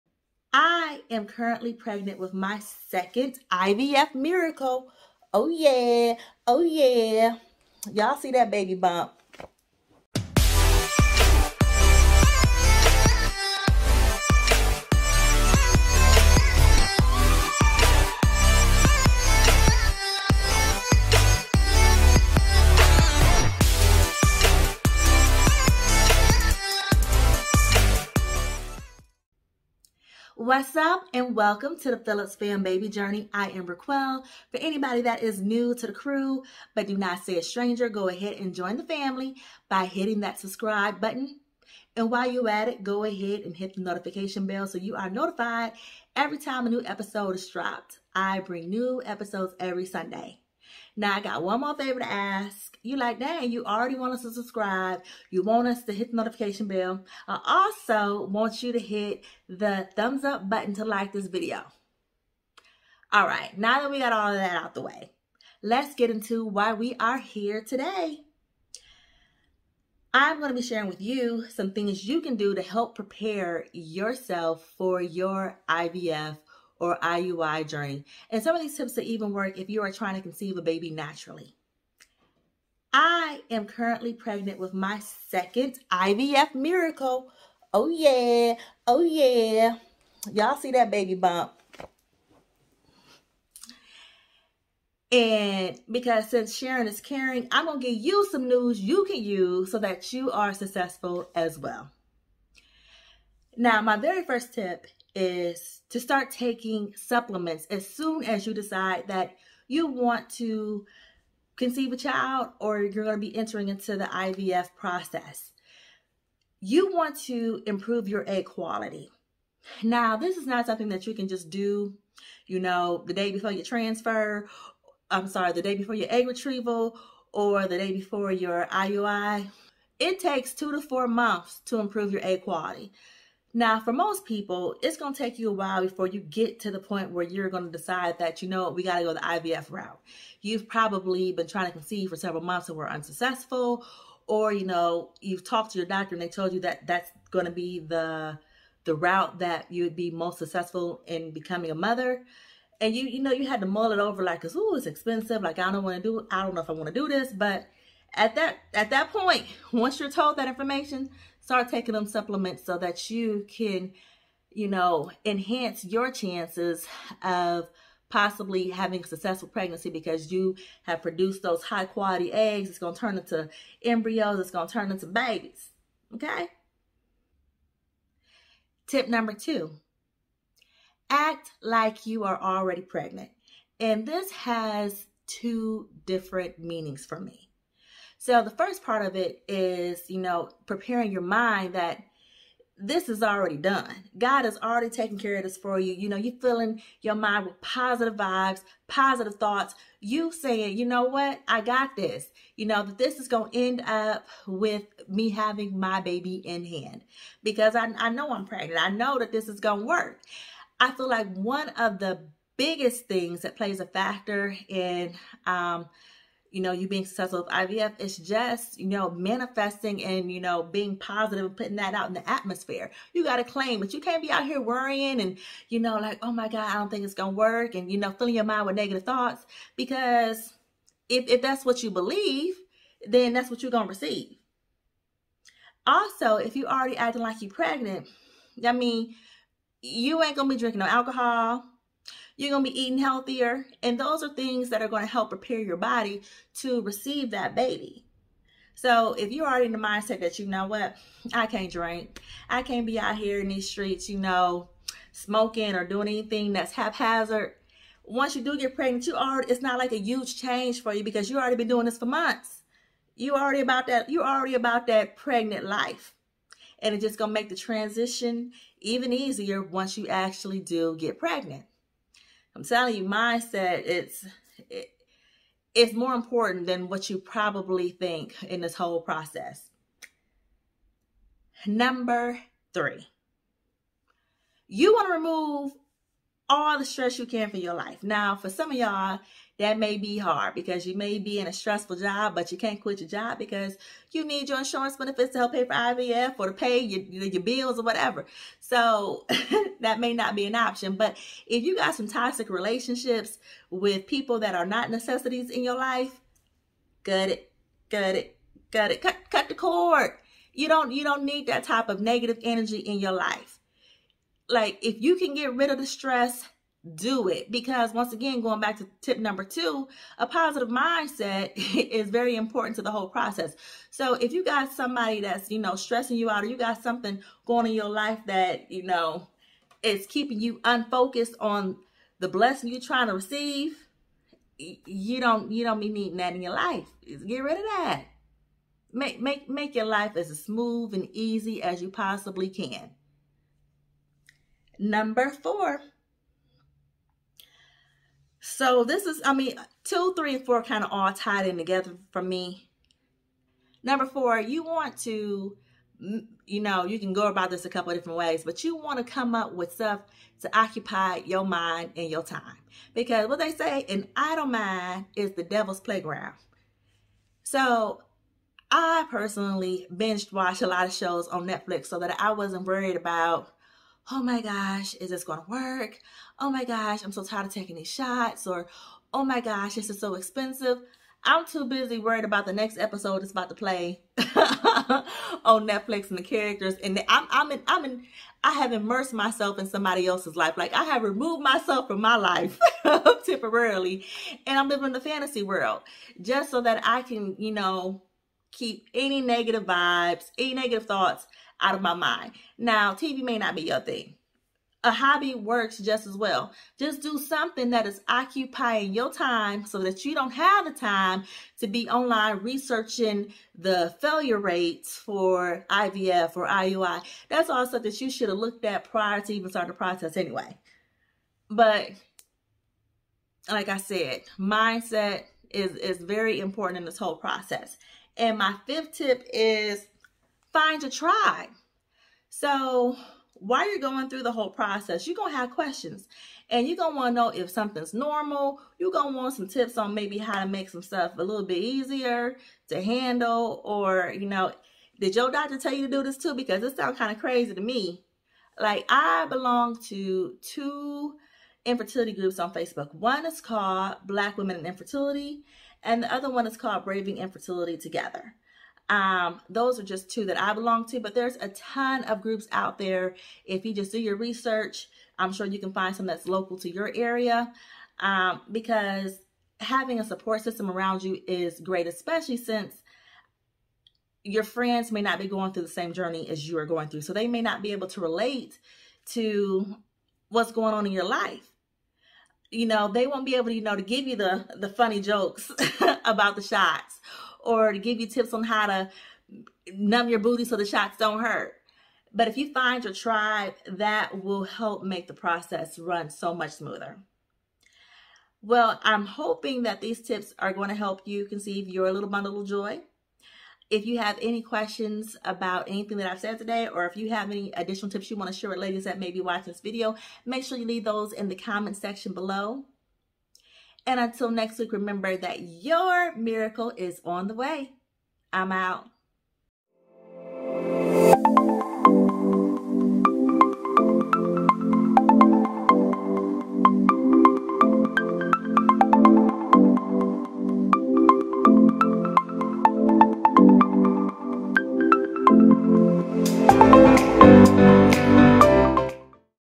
Tips to Prepare for IVF | IVF Transfer Tips
These tips to prepare for IVF also will work for those preparing for an IUI or trying to conceive. I get asked all the time How do you prepare for IVF or what can I do to increase my chances of IVF being successful. I give 5 tips for IVF preparation.